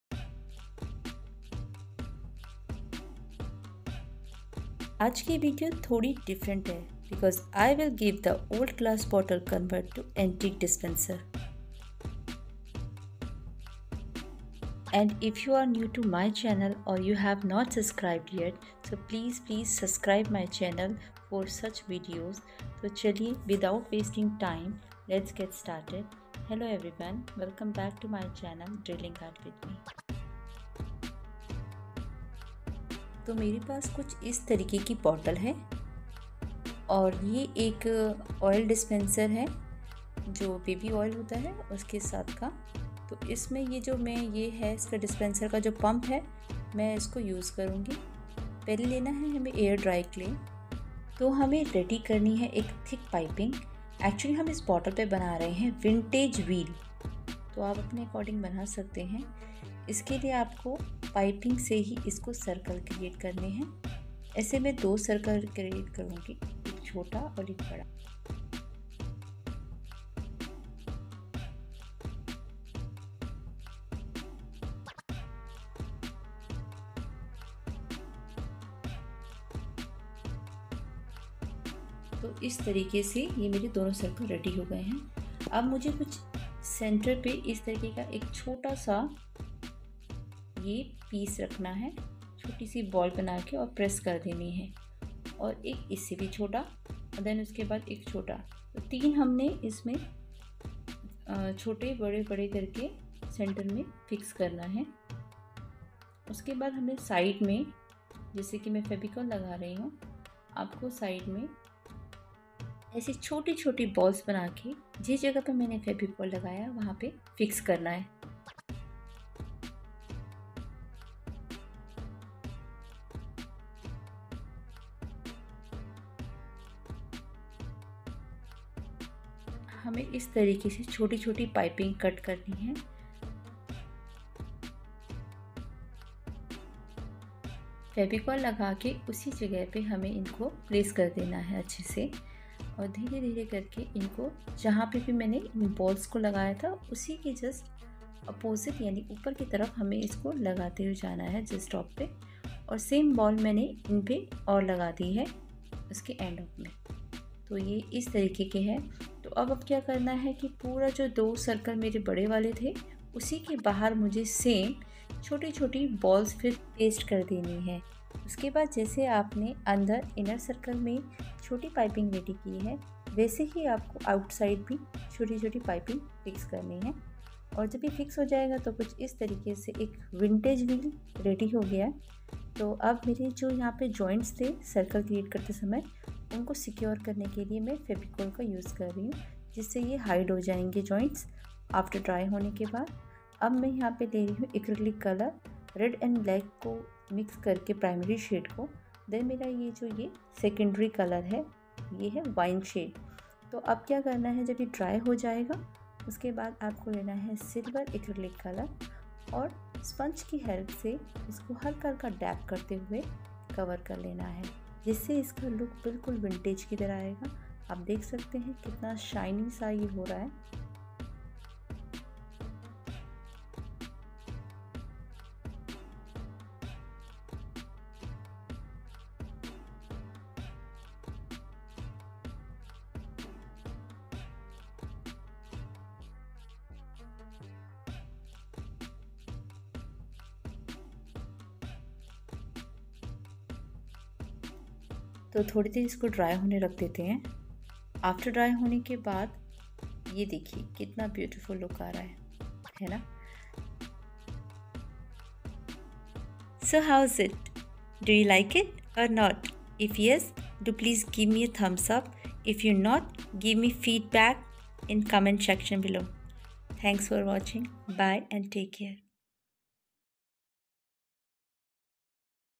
antiquesoapdispenser || how to make soap dispenser
Today's video is something different because I will give the old glass bottle convert to antique and unique shop dispenser